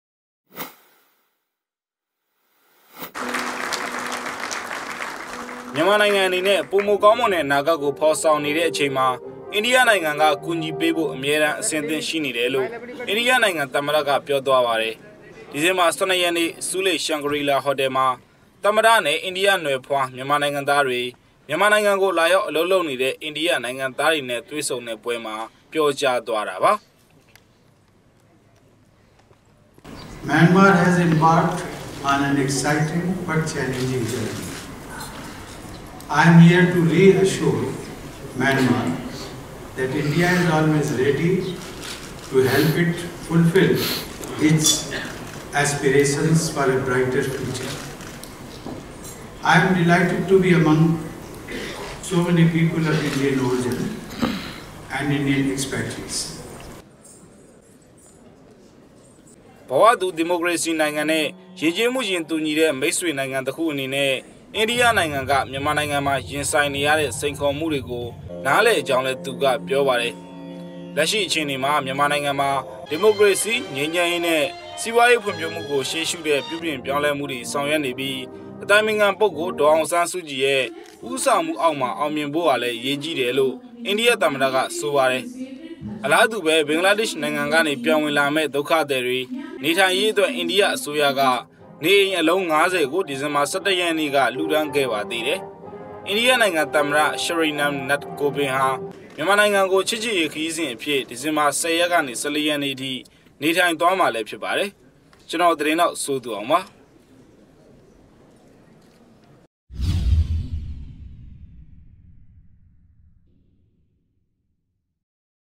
ျမန္မာႏိုင္ငံကို ကူညီရန္ အၿမဲဆင္သင့္ရွိေနဟု အိႏိၵယသမၼတ ေျပာၾကား
ျမန္မာႏိုင္ငံအေနနဲ႔ ပိုမိုေကာင္းမြန္တဲ႔အနာဂတ္တစ္ရပ္ကို ေဖာ္ေဆာင္ေနတဲ႔အခ်ိန္မွာ အိႏၵိယႏိုင္ငံက ကူညီေပးဖို႔ အၿမဲတမ္း အဆင္သင့္ရွိေနတယ္လို႔ အိႏိၵယႏိုင္ငံသမၼတက ေျပာသြားပါတယ္။